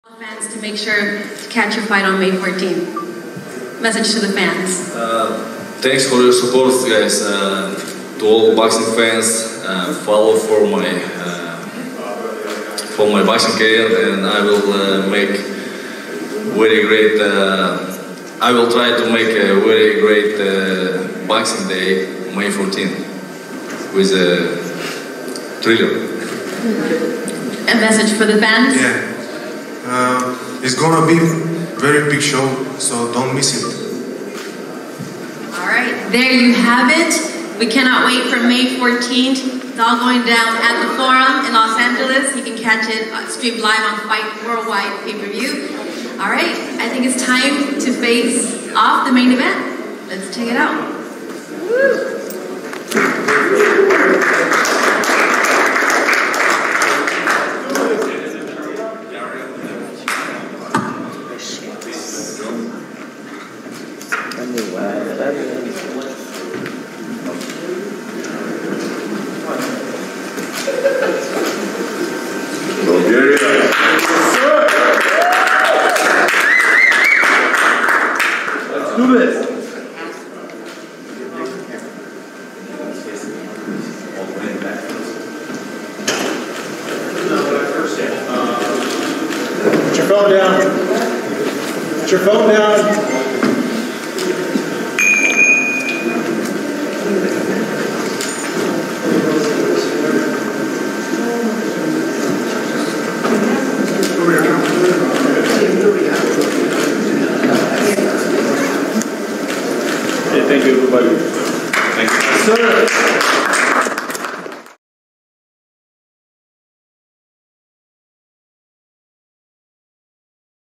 SERGEY KOVALEV STARES DOWN TERVEL PULEV; SIZES UP BIGGER CRUISERWEIGHT CHALLENGE
FightHype.com was on the scene in Los Angeles, California for the kickoff press conference to officially announce TrillerVerz 5, featuring Sergey Kovalev vs. Tervel Pulev, Kubrat Pulev vs. Andrey Fedosov, Evan 'Yung Holy' Holyfield, and match-ups featuring all 3 of Fernando Vargas' sons. You don't want to miss what went down. Check it out!